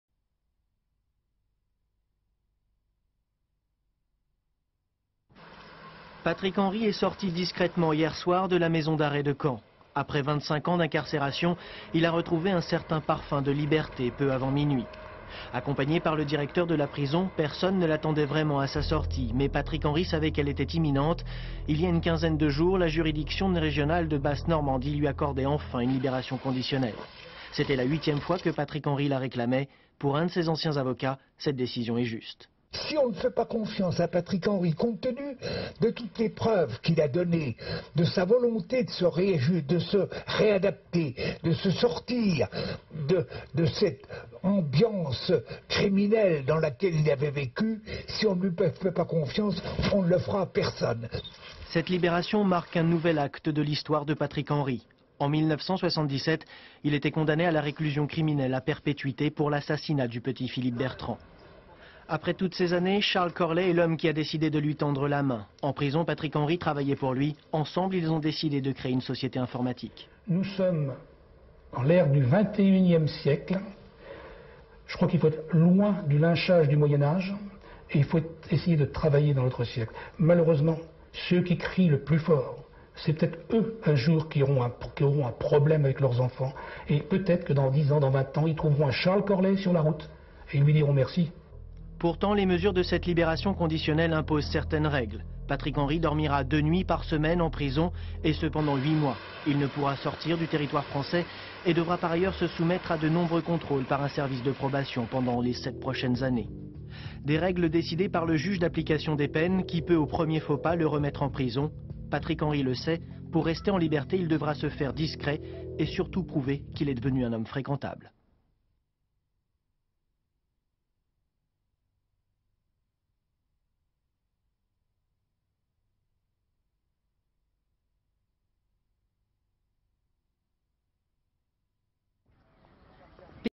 Patrick Henry en liberté conditionnelle | Archive INA
15 mai 2001
Patrick HENRY, condamné en 1977 à la prison à perpétuité pour le meurtre d'un enfant, est sorti cette nuit de la prison de CAEN après 25 ans de détention. Il a obtenu une mesure de libération  conditionnelle, prononcée le 26 avril. Aujourd'hui âgé de 57 ans, il a trouvé un travail à CONDÉ-SUR-NOIREAU, dans le Calvados, chez un imprimeur. Images d'archive INA